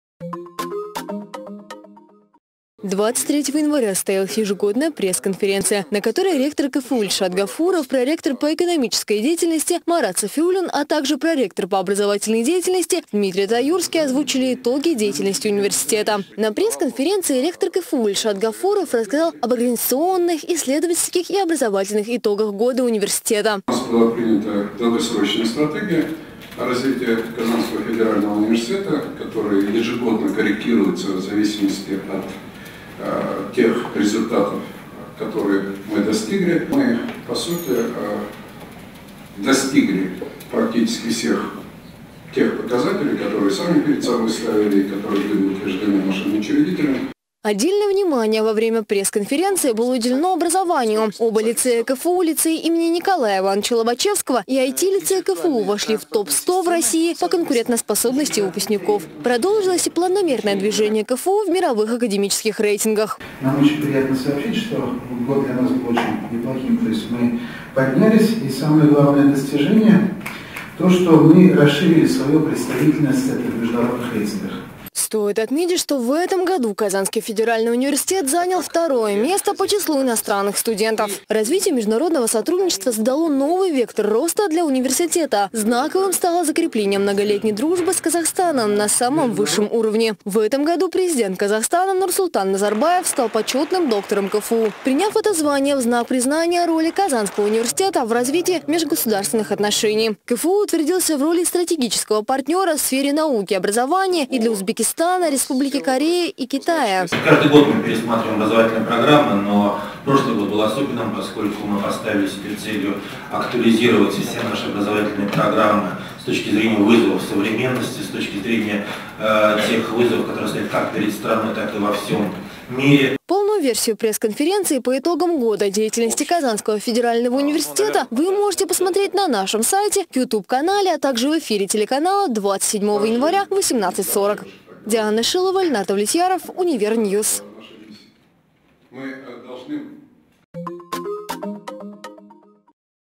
Итоги года КФУ
Прошедший 2018 год стал для КФУ насыщенным на события. О том чего достиг университет за прошедший год на ежегодной пресс - конференции рассказал ректор КФУ Ильшат Гафуров.